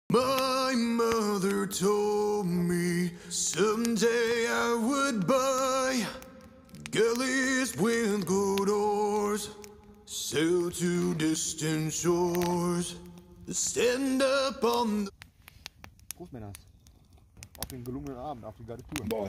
Bushcraft, Lagerleben Overnighter im Schnee.
Abends am Feuer im Schnee auf der Wintertour